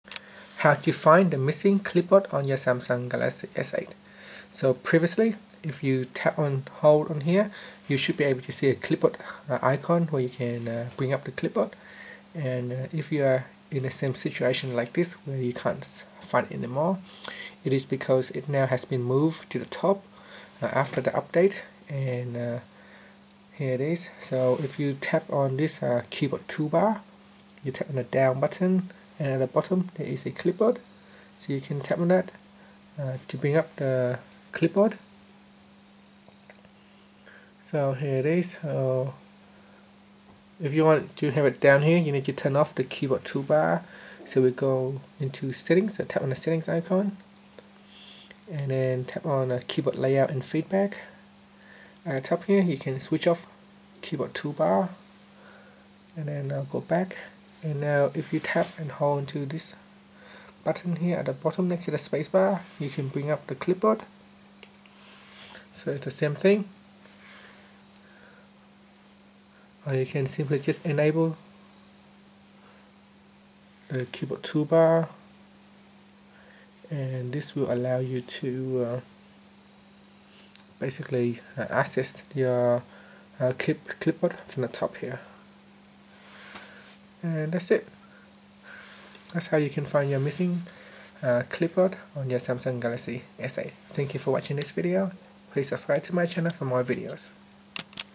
Samsung Galaxy S8: How to Find the Missing Clipboard on the Keyboard
Learn how you can find the missing clipboard on the keyboard on Samsung Galaxy S8.